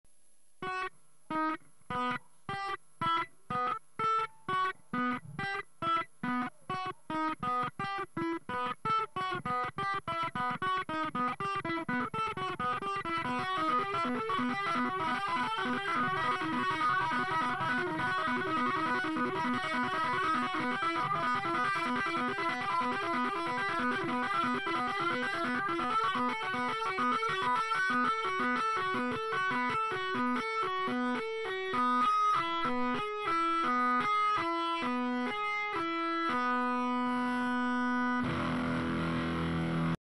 Angus Young Solo Cover
A short Angus Young Solo played by me.

playing for 6 months so don't expect too much ;-)

RIP Bon! Rock on!!